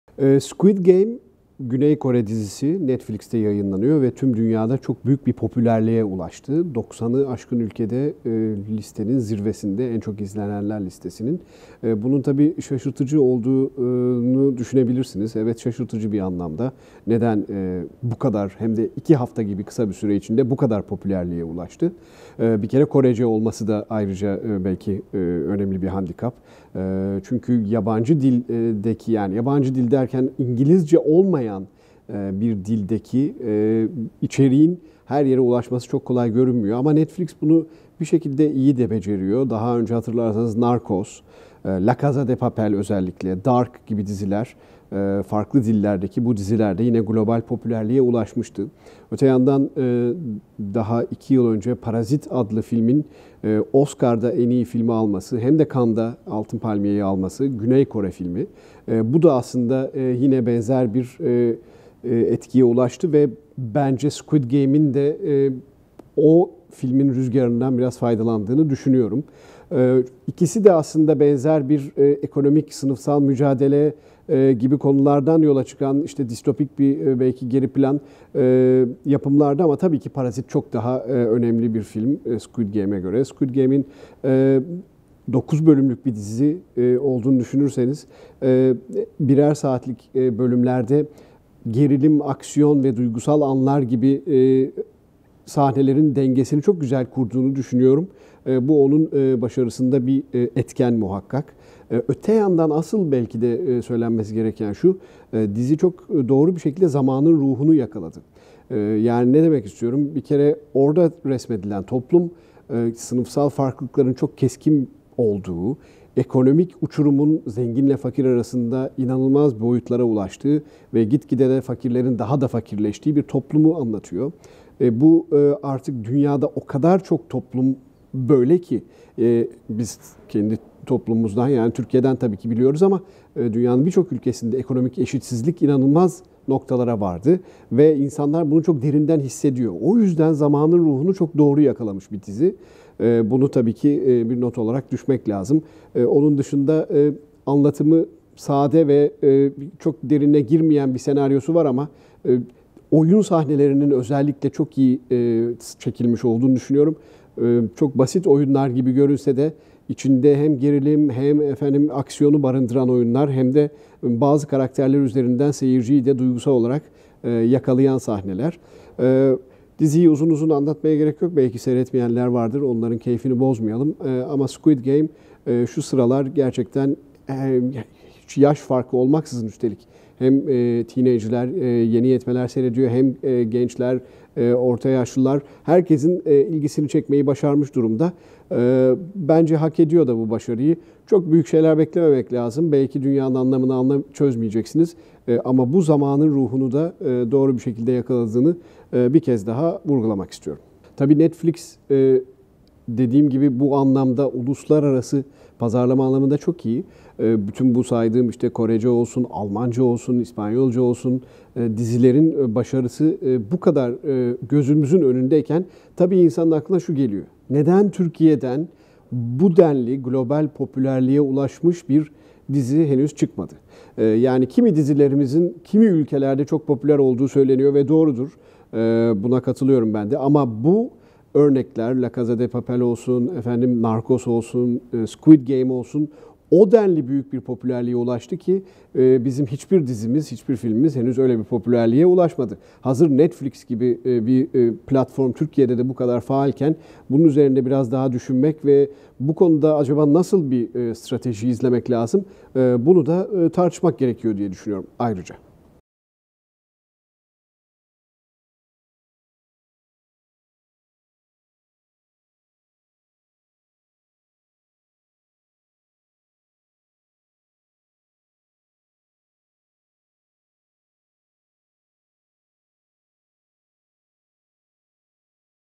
Dünyayı kasıp kavuran Squıd Game analizi | Squıd Game nedir? Squıd Game konusu nedir?
Dünyayı kasıp kavuran Squıd Game dizisini Cumhuriyet Pazar kültür sanat editörü Emrah Kolukısa değerlendirdi.